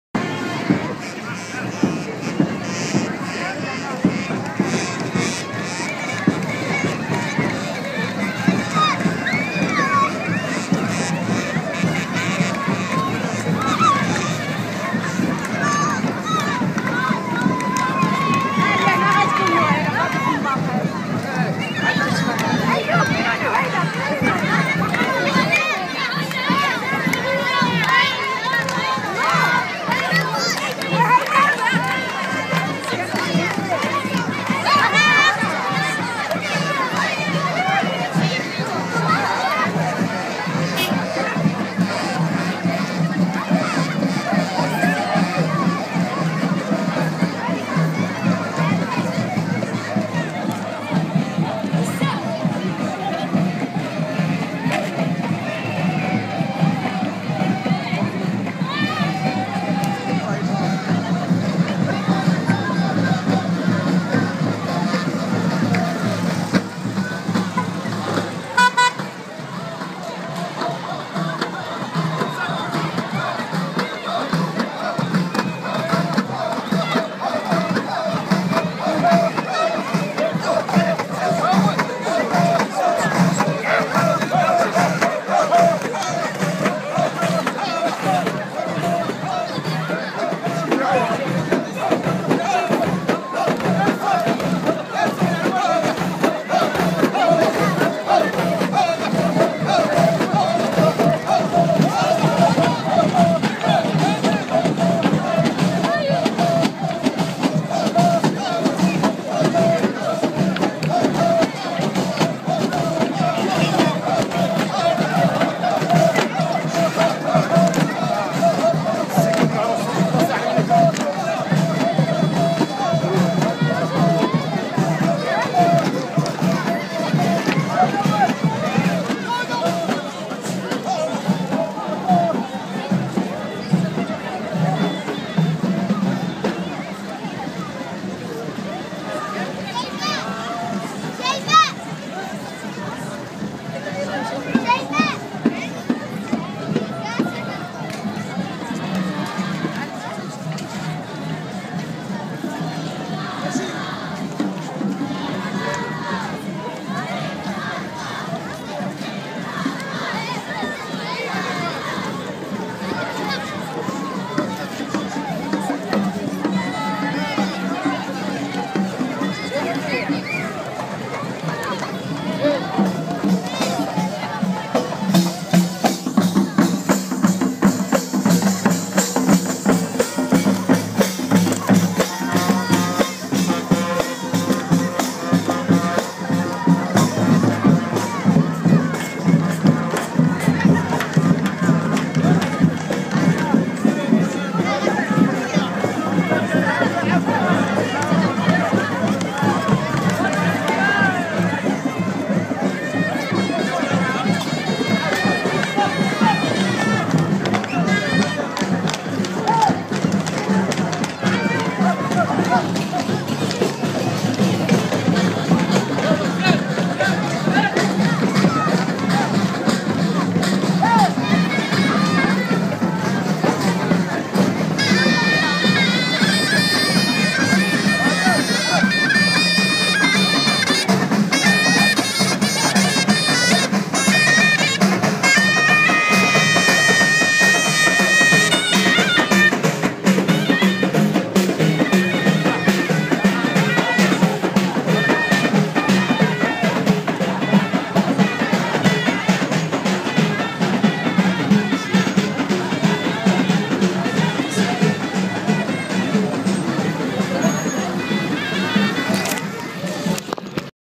المهرجان الجهوي للطفل الخميسات
انطلاق فعاليات المهرجان الجهوي للطفل في نسخته الأولى بالخميسات، بكرنفال جاب أهم شوارع المدينة عشية الجمعة 10 ماي 2013.